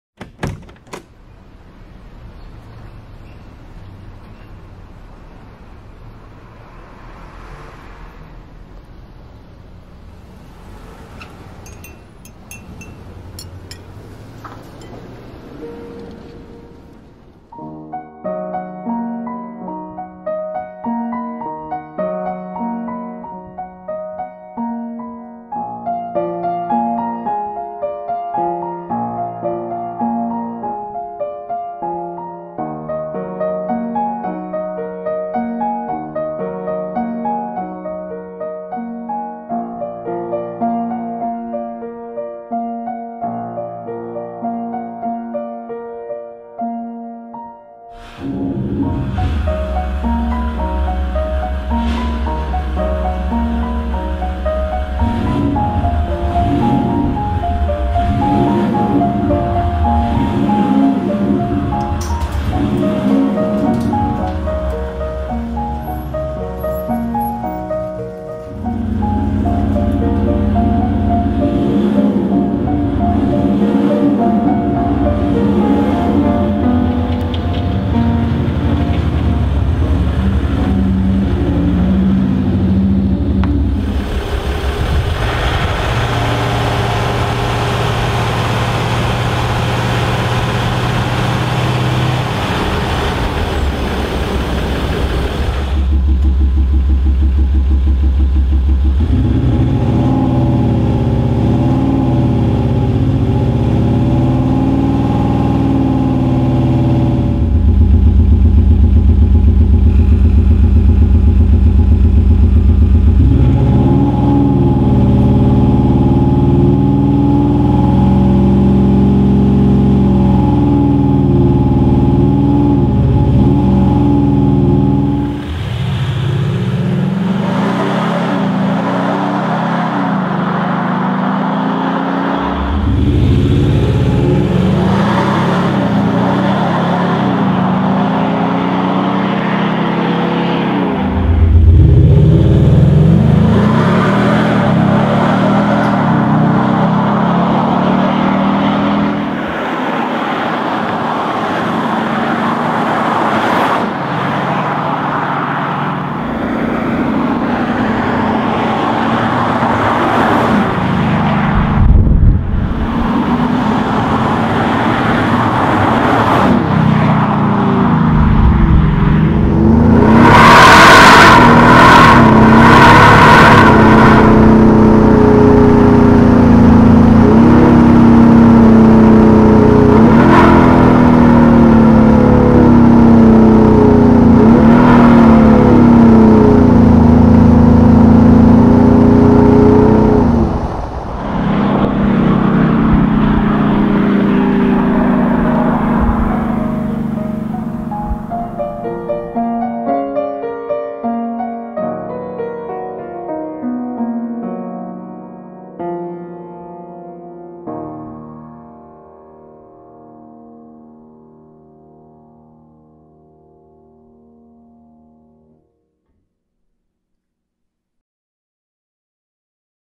Maserati Levante The Art of Sound by Quicksilver Exhausts
We make a V6 Diesel sound like a V8 Muscle Car using our Sound Architect electronic device. The device plugs into the ECU and works with engine revs and throttle position to give a real V8 sound which is emitted from the tail pipes. the device is also fully customisable using our mobile phone APP.

Video of Woman: Videezy.com